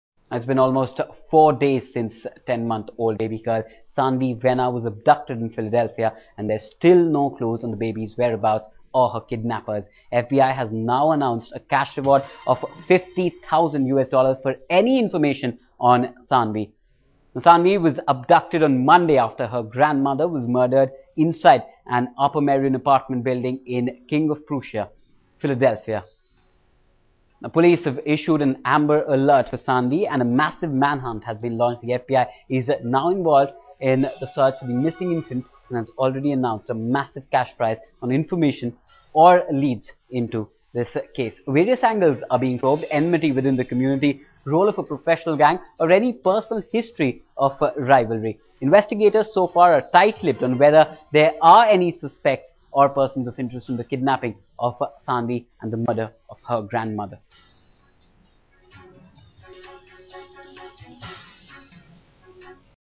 FBI announces award of $50,000 for information on baby Saanvi
The Federal Bureau of Investgation has joined the search for 10-month-old baby girl Saanvi Venna, who was kidnapped after her grandmother was murdered in an apartment block, and offered a reward of of $50,000 for information on her.